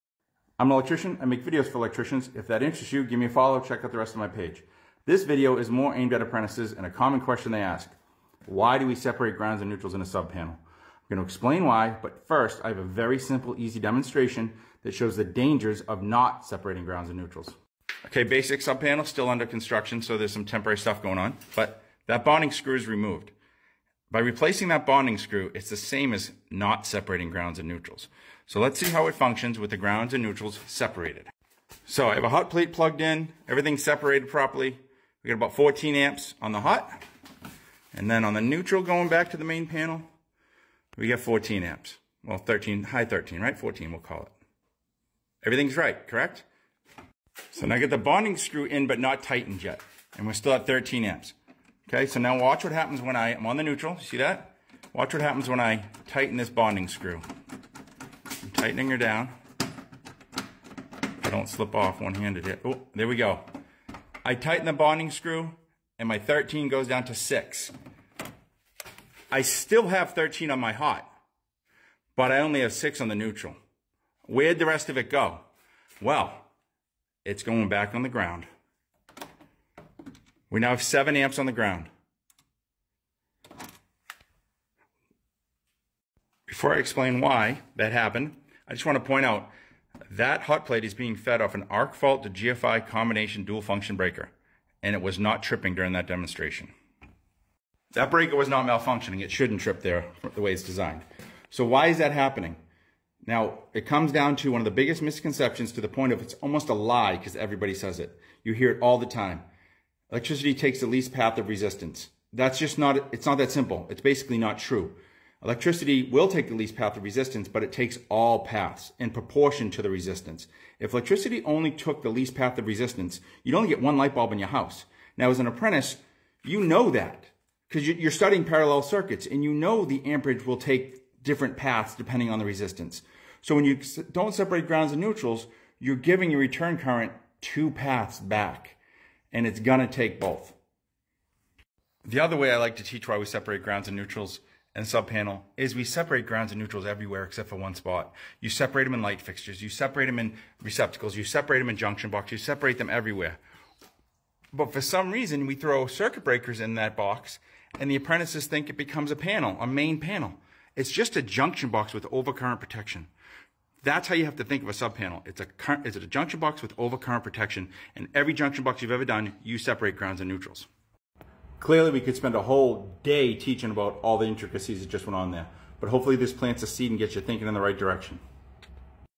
CLICK HERE FOR FULL VIDEO why do we separate grounds and neutral in a sub panel
what happens if you do separate ground and neutral in a sub-panel